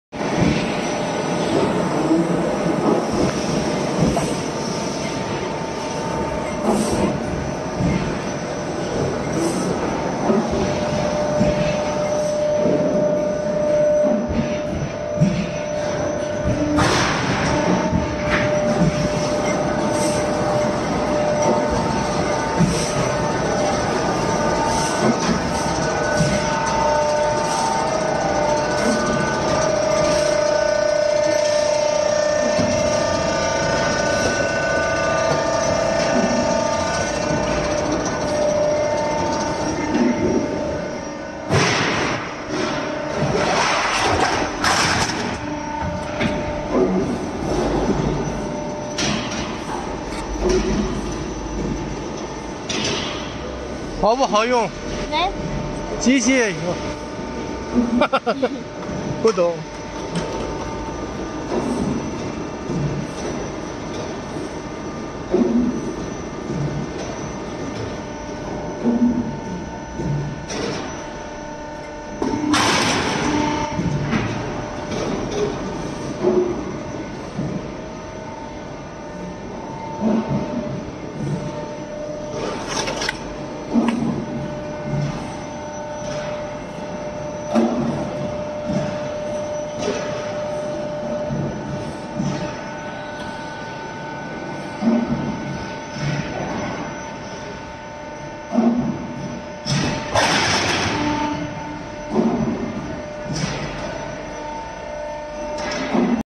ZHONGTUO Roof Sheet Machine Factory Visit#
This is our Korea customer's factory. All the roof sheet machines you saw are made from us.